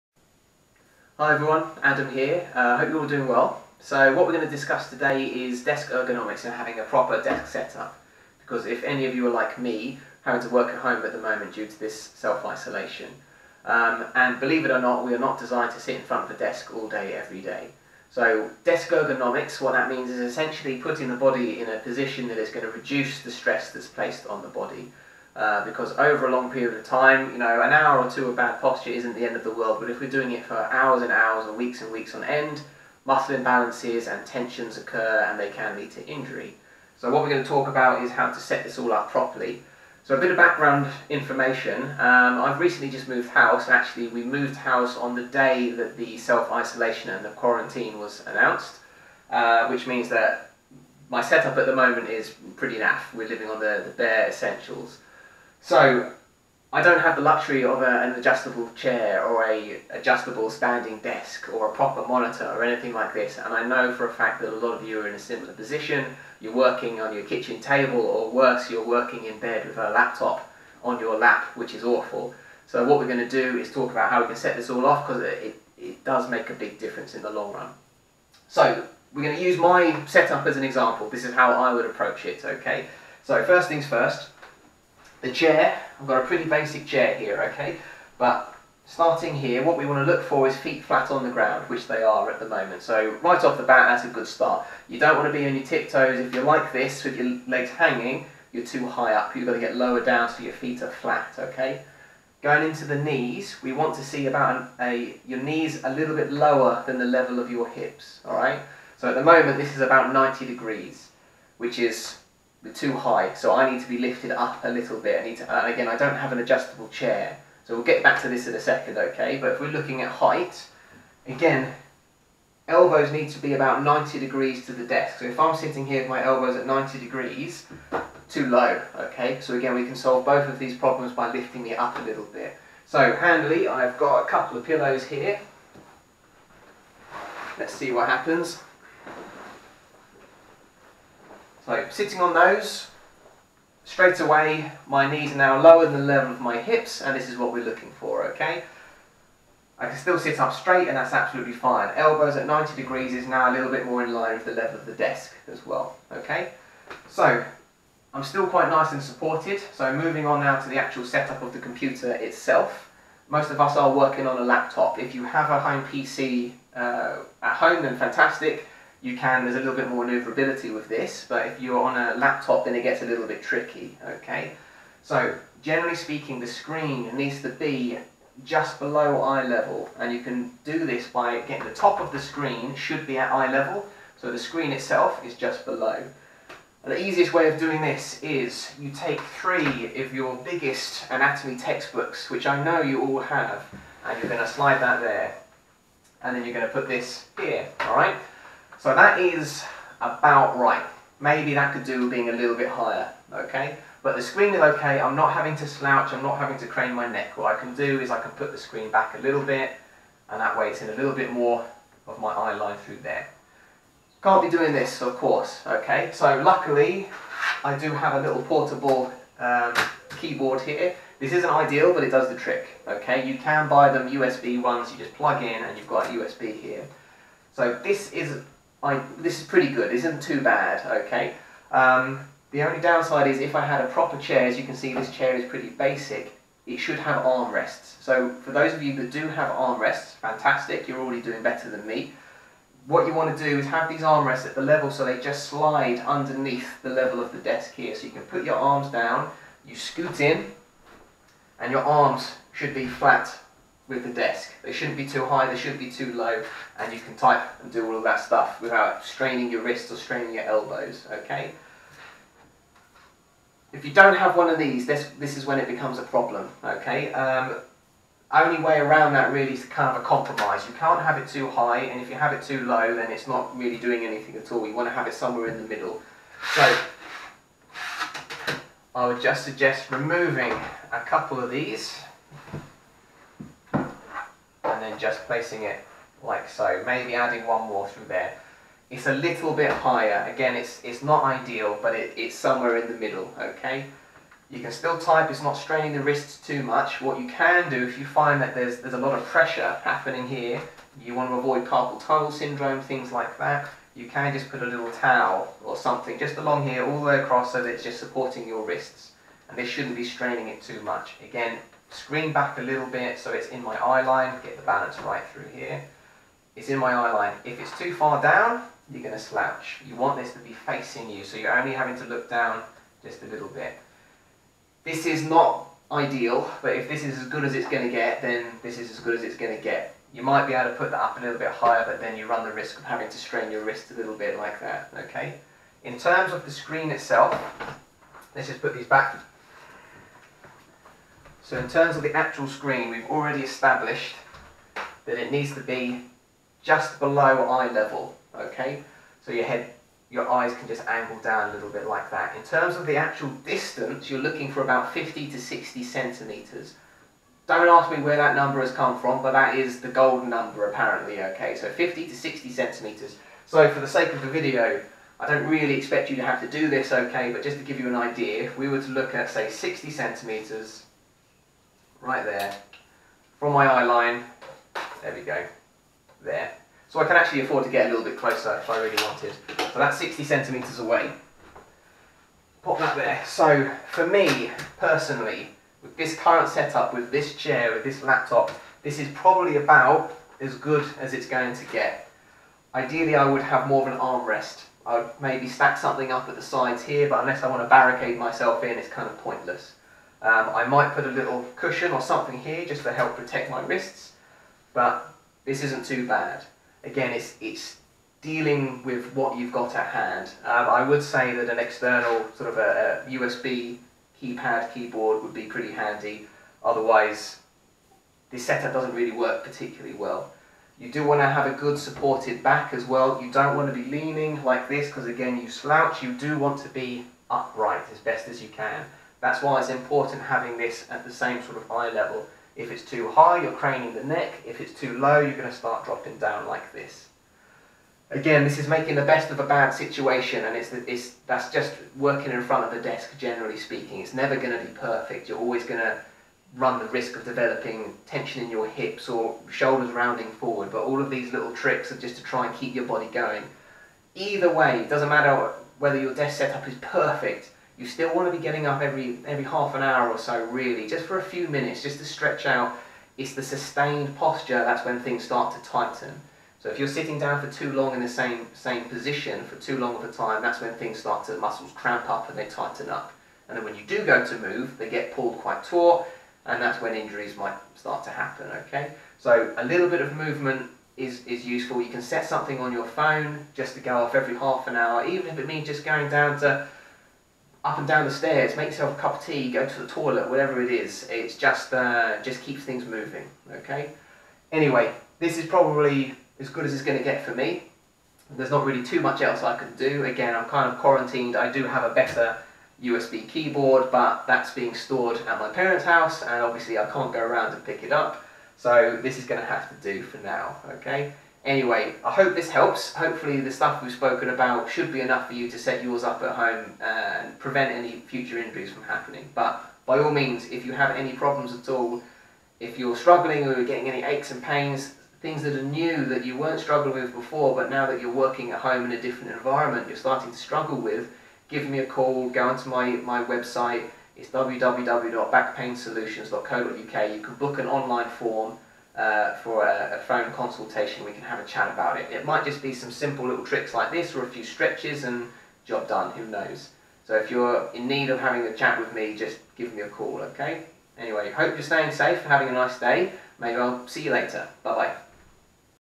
Adam Sealey Osteopathy - How to set up your desk ergonomically!
A lot of us find ourselves in a position where we need to work at home but don't have the luxury of a high end office chair or a suitable desktop PC.
In this video I will walk through the steps that you need to take in order to correctly set up your home office by using my own work space as a real life example.